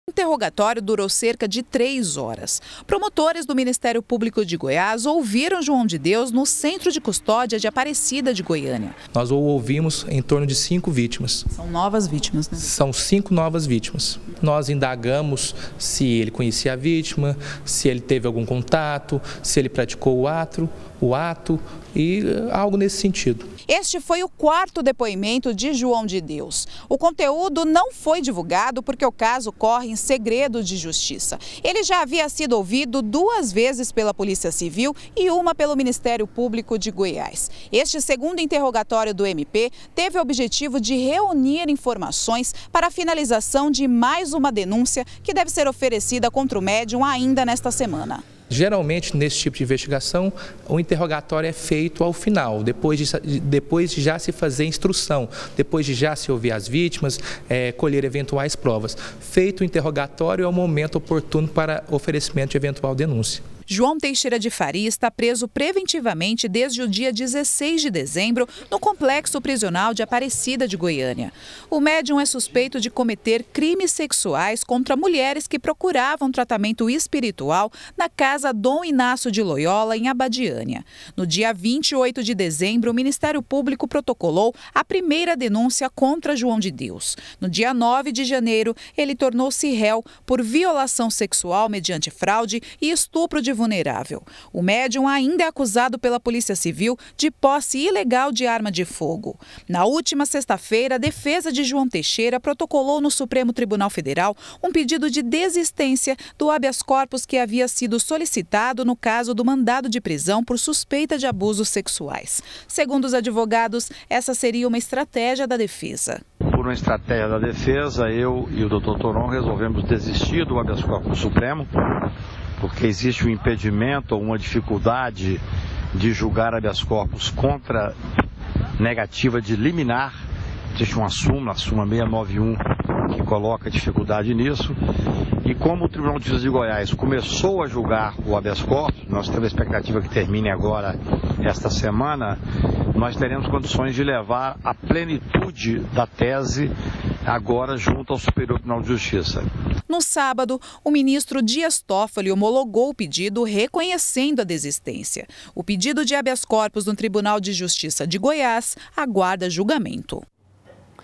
CASO JOÃO DE DEUS: MÉDIUM É OUVIDO PELO MINISTÉRIO PÚBLICO
João de Deus foi ouvido ontem pelo Ministério Público. O objetivo é reunir informações para finalizar mais uma denúncia contra o médium ainda hoje. Este foi o quarto depoimento dele depois que várias vítimas resolveram falar dos casos de abuso sexual.